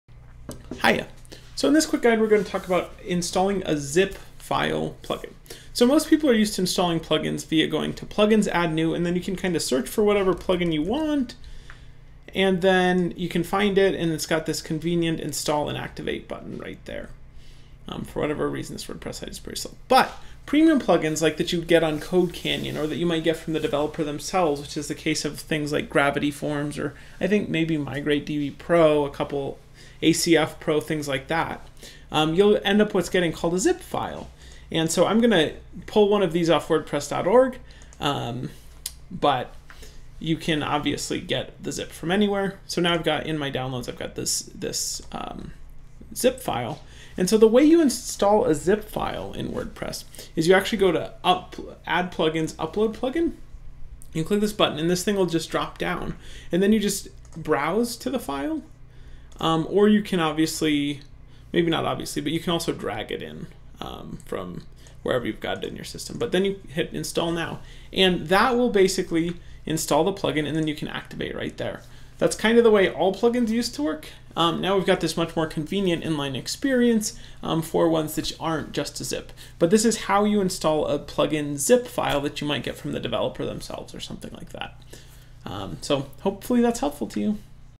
Uploading a Plugin to WordPress Made Easy
This video is a step-by-step guide on how to install a plugin zip file in WordPress.

Zip files are simply compressed folders of other files in a single new file. Since a plugin is just a set of (mostly PHP) files, you just need to hand WordPress that compressed zip file and it’ll decompress it and put it in the right place for you.  This also applies to themes, but with a couple of extra steps.

WPShout: In-Depth WordPress tutorials for Developers.